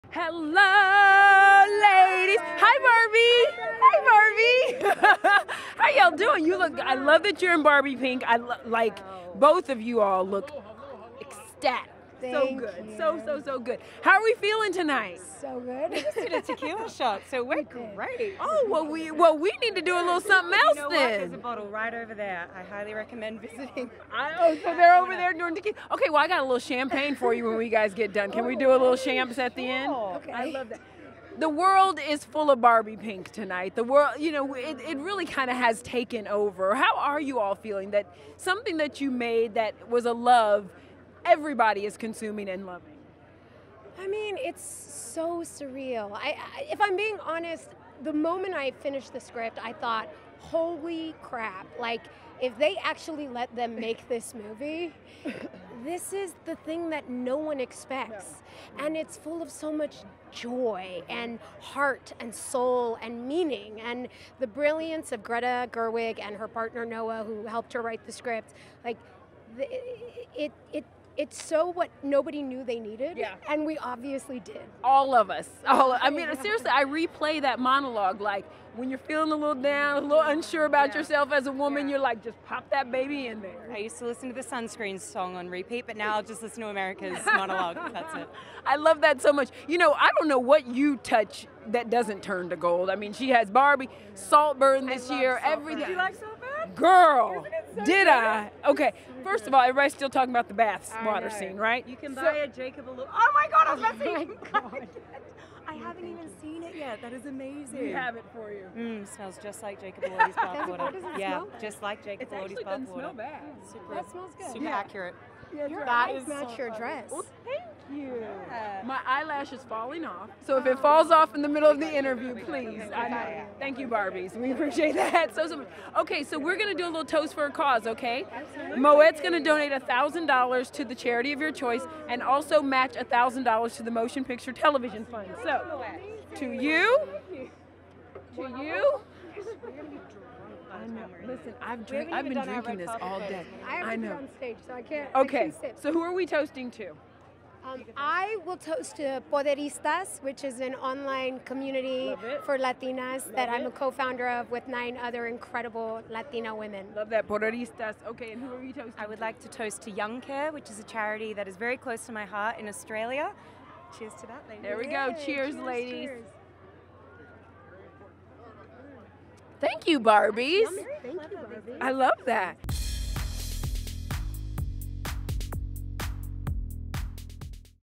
Margot Robbie FREAKS OUT Over Jacob Elordi’s Saltburn Bathwater Candle (Exclusive)
Margot Robbie and America Ferrera chat with ET on the red carpet at the 2024 Golden Globe Awards, airing on CBS. The 'Barbie' co-stars reflect on 'surreal' experience filming their blockbuster film. Plus, Margot, a producer on the film 'Saltburn,' smells star Jacob Elordi's viral bathwater-scented candle inspired by the movie!